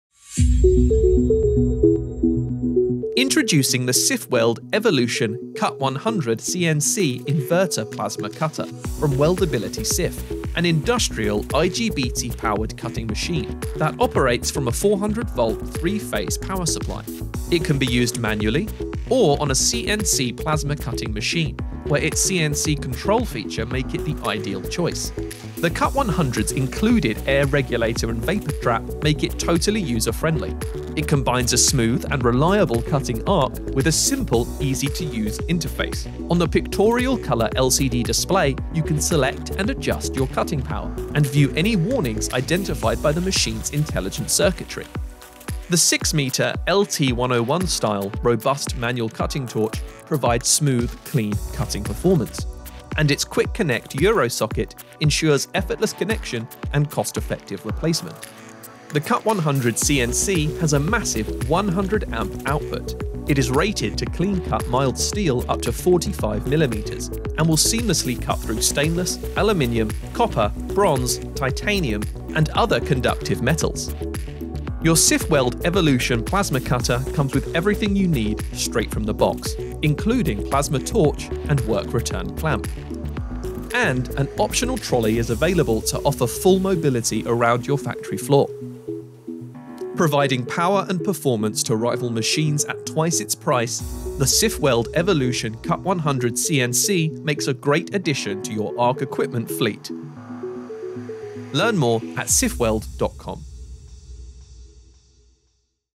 sifWELD Evolution: Cut100 CNC - 100Amp Manual Plasma Cutting Machine
The sifWELD Cut100 CNC is a professional 100 Amp system designed for efficient cutting in the heavy fabrication, manufacturing, demolition and general maintenance industries. With 100% duty cycle a CNC connection can be connected to a plasma cutting table for precision automatic cutting if required.

Featuring IGBT inverter technology, and is ideal to cut up to 60mm thick steel. The 6m cutting torch ensures exceptionally smooth, clean cuts, and comes with a quick-connect euro connector. Cutting cut mild and low-alloy steels, stainless, aluminium, copper, titanium and nickel alloys.